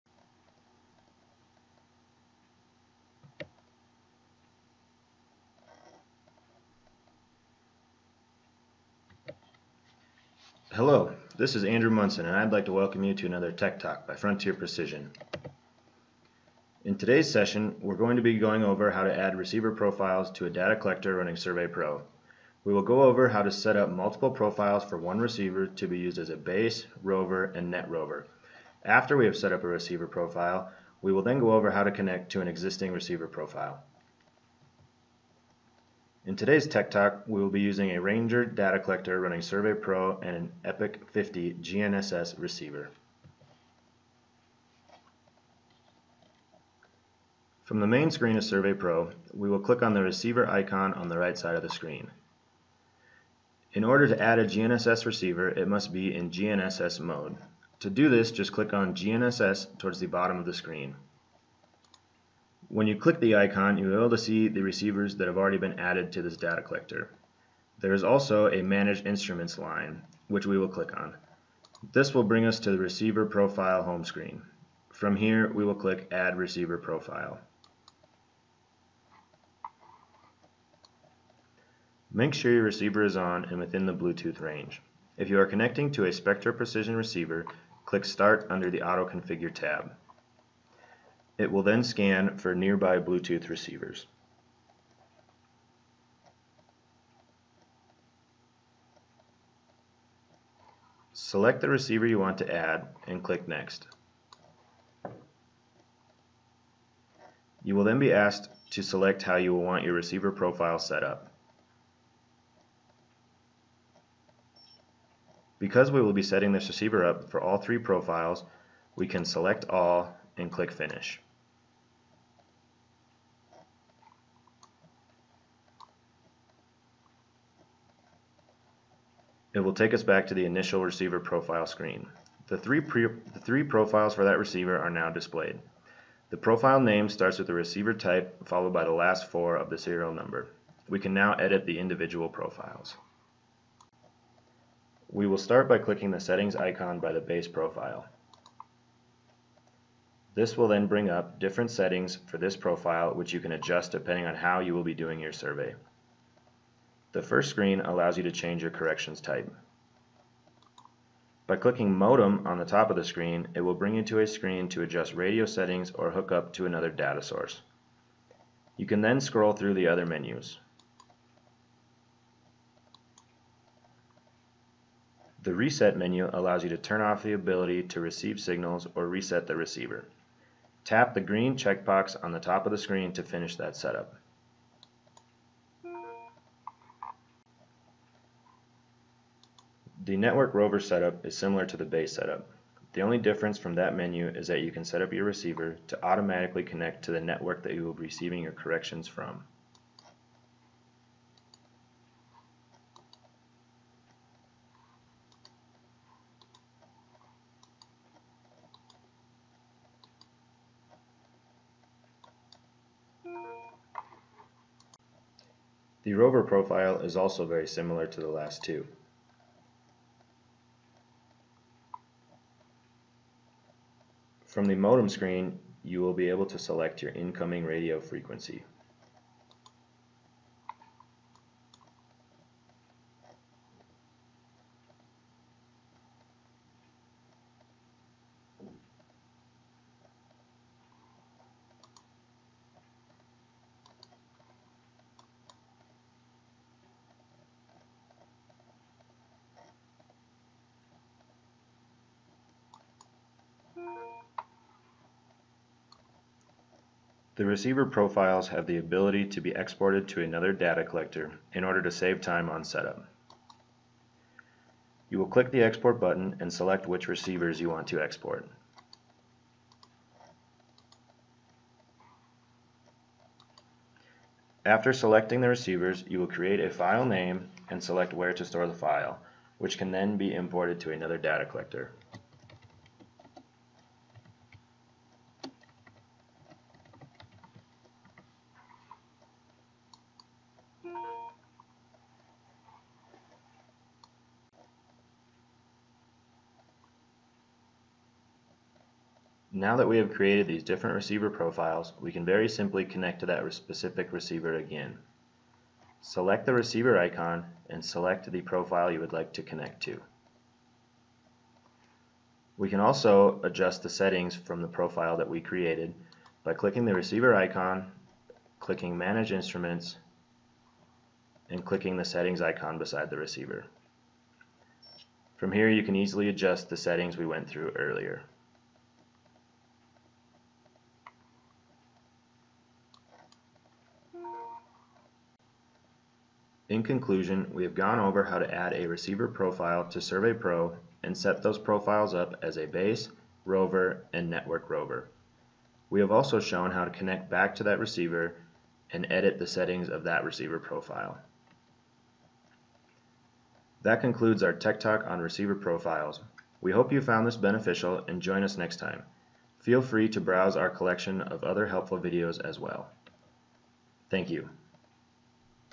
Adding a receiver profile to Survey Pro
In this video we demonstrate how to add a receiver profile to Survey Pro via a Bluetooth connection.